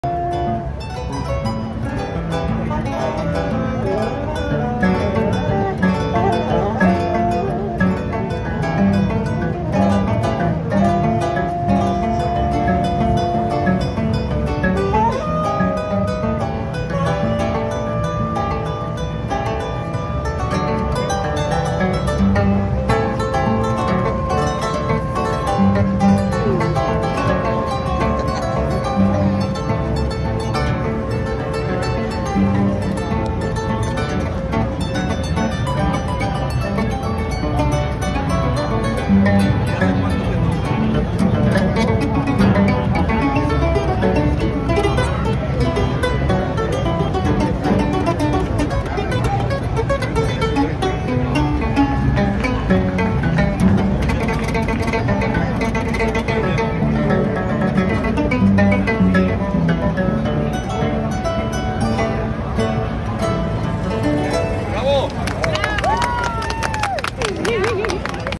Jose Manuel con la música llanera en las calles de New York
Al parecer les gusta la música Venezolana policía de NY